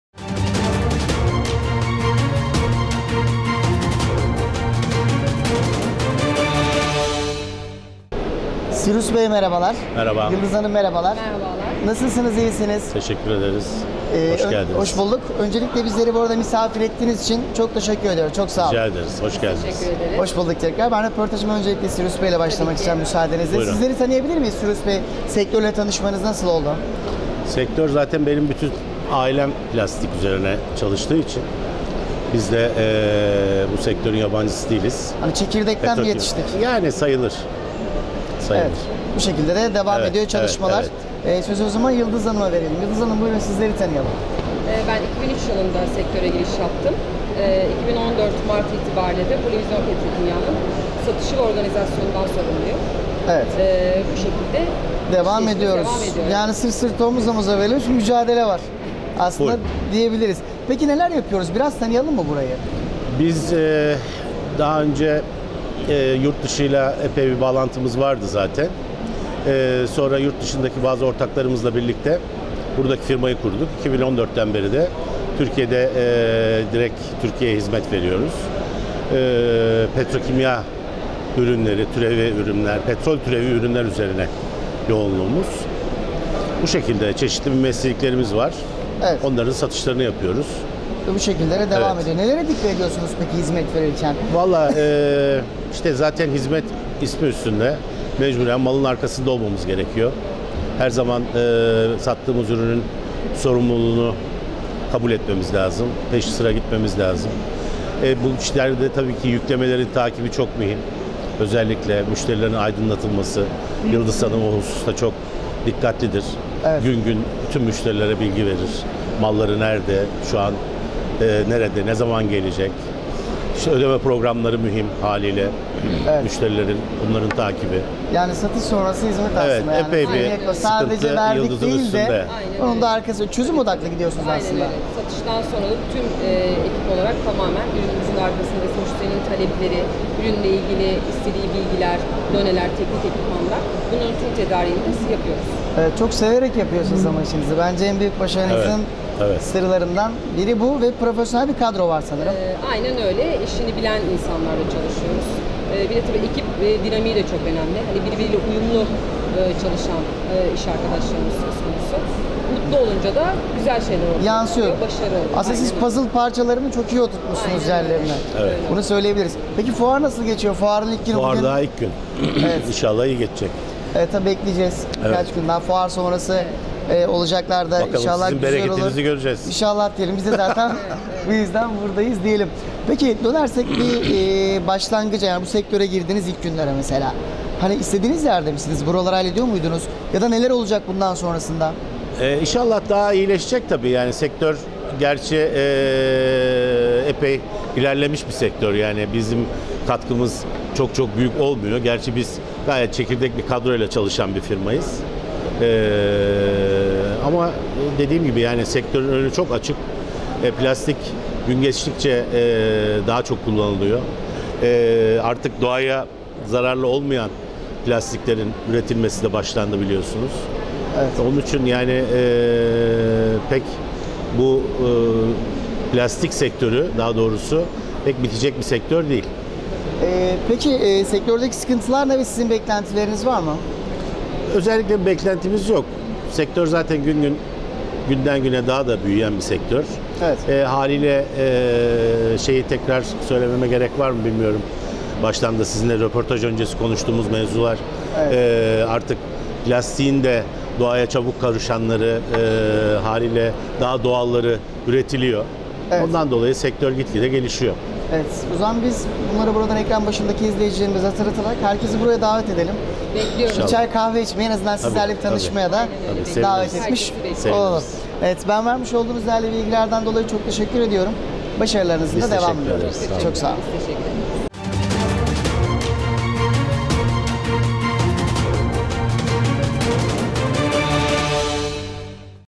POLİVİZYON PETROKİMYA SANAYİ -FUAR TV
İstanbul Ataşehir'de Petro kimya sektöründe faaliyette bulunan "POLİVİZYON PETRO KİMYA " firmasının Yönetim Kurulu Başkanı Sirus LEKI ile muhabirimiz Sinan Topçuoğlu Fuar alanında firmanın standında görüştü.